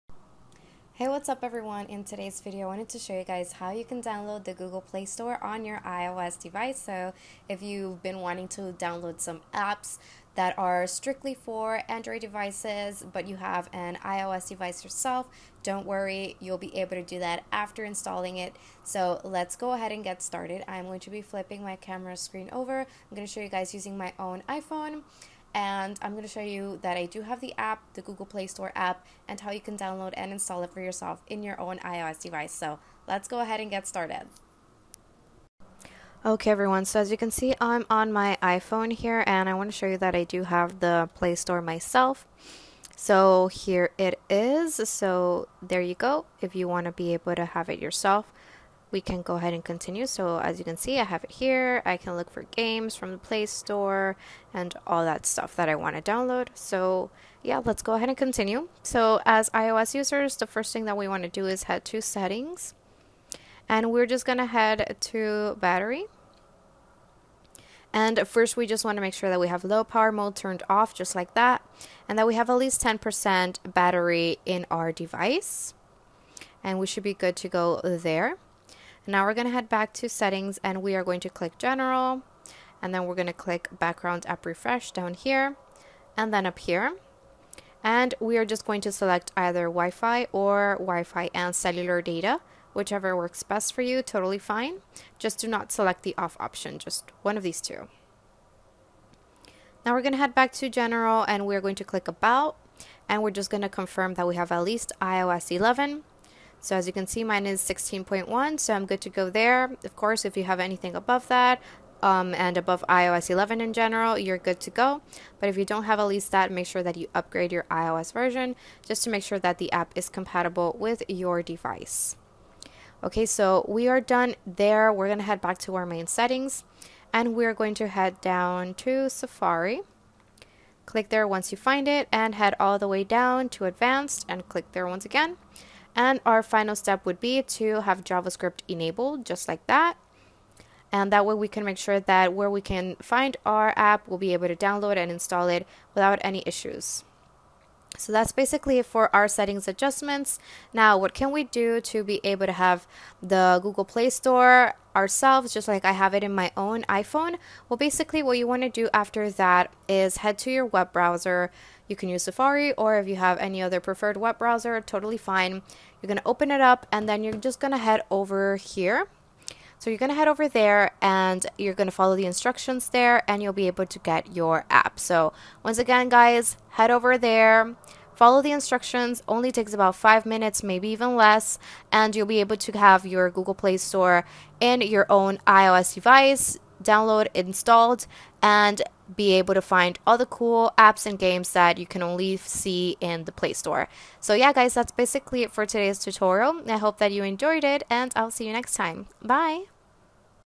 🔴 How to Install Google Play Store on iPhone 📱 *EASY TUTORIAL*
how to get google play store on ios/iphone/ipad - google play store install for iphone, get apks on iphone !